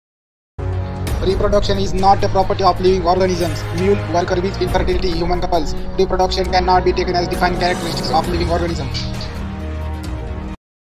reproduction is a not a property of living। interesting textbooks points
today interesting textbook point is
reproduction is not a property of living 👍 interesteing textbooks point this .